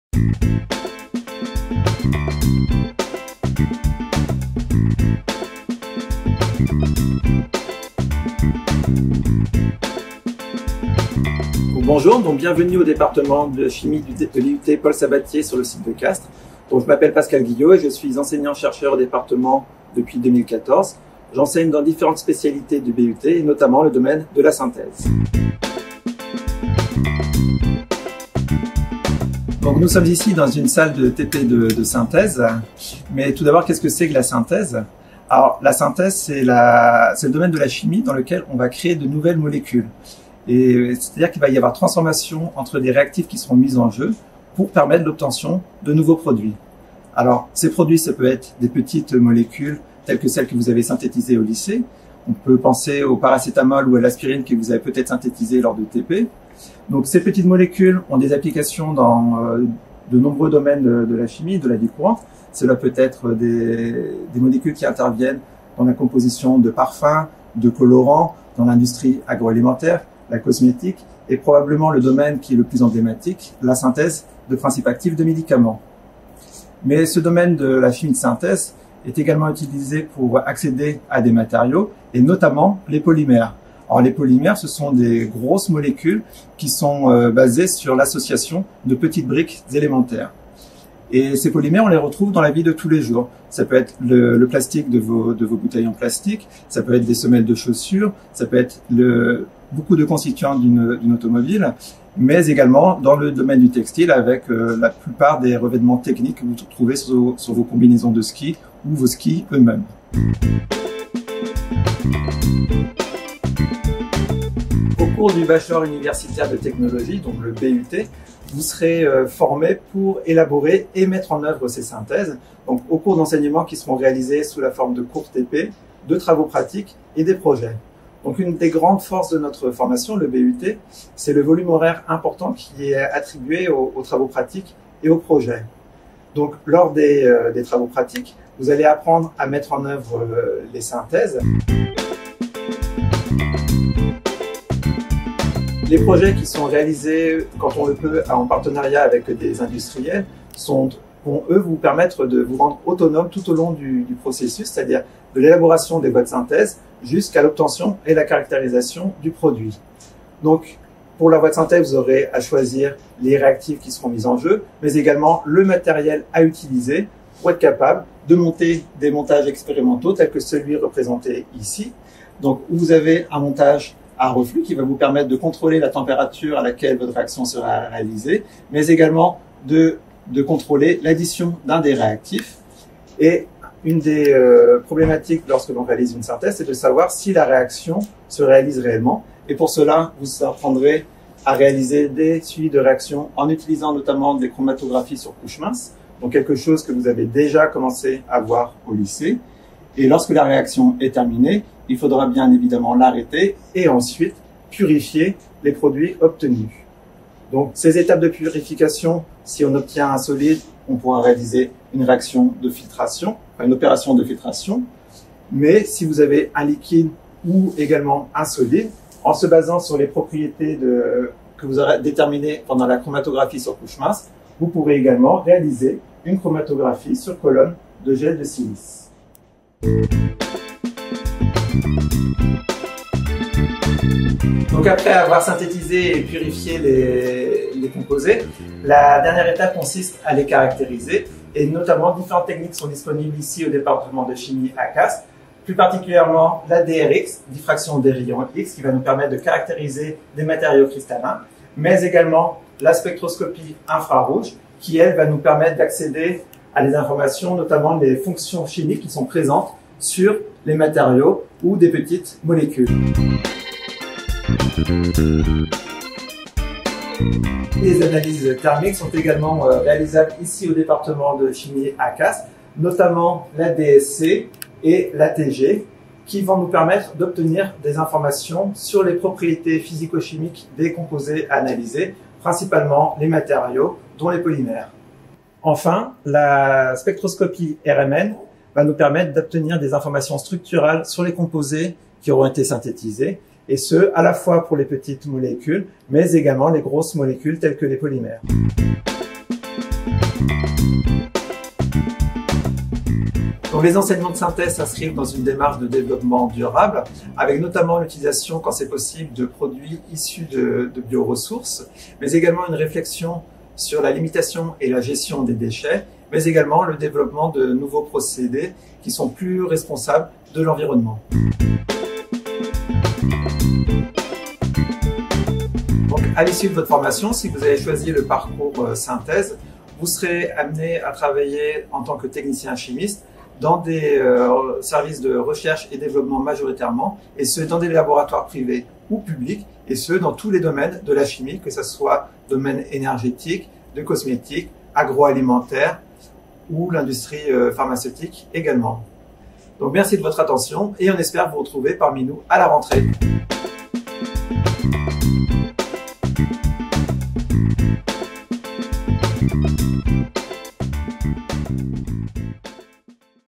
Découvre ta formation : la chimie de synthèse
Pascal Guillo, enseignant au département de formation Chimie à Castres, vous présente et vous explique ce qu'est la chimie de synthèse.